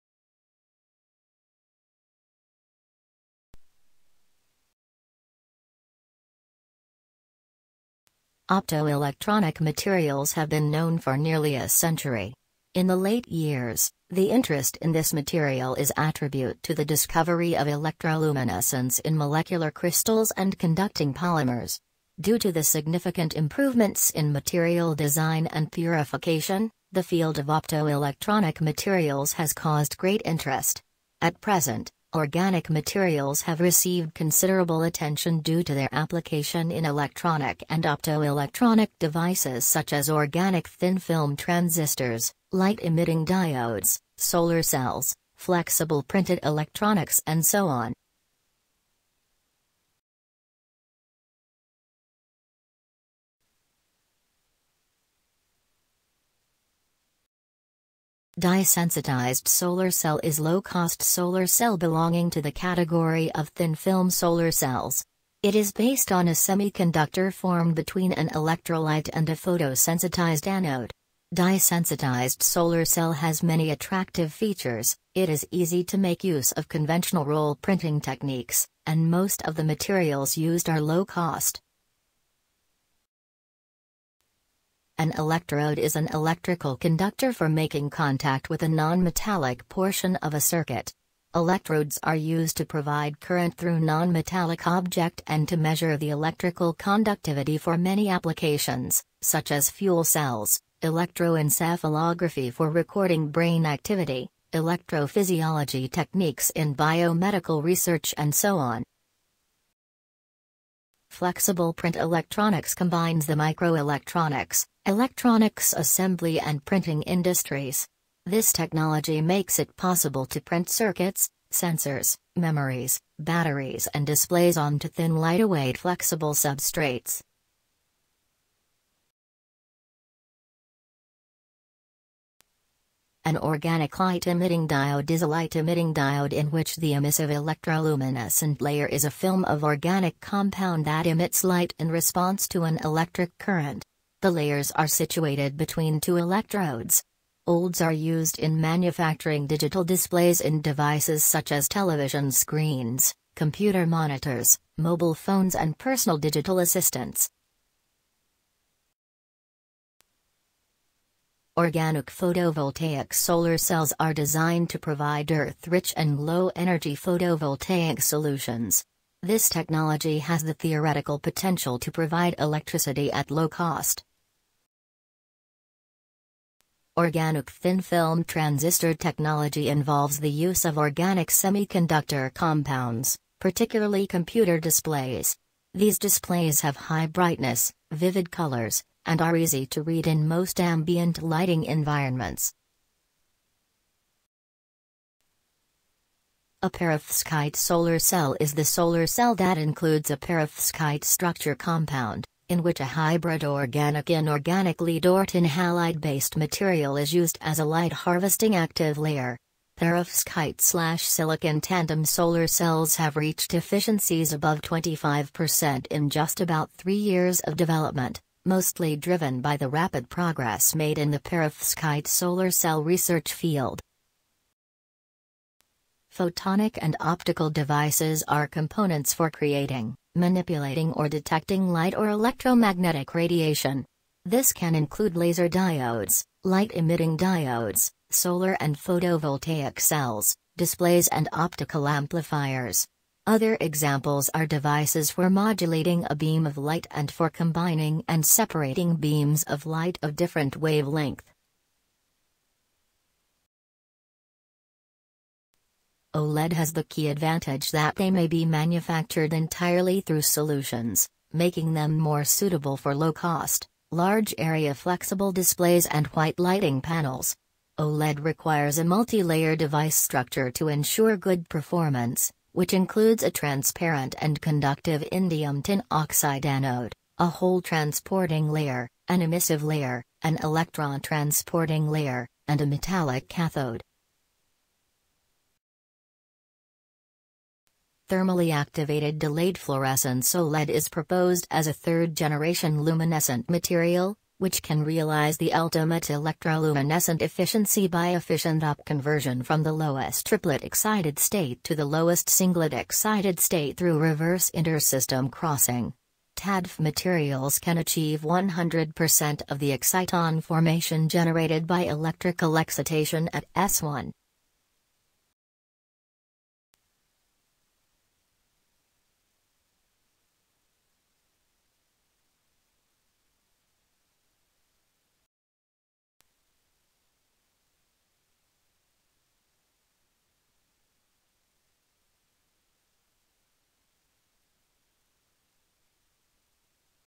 Optoelectronic Materials
Introduction of  all kinds of organic materials and their applications. More information about Optoelectronic Materials is on Alfa Chemistry.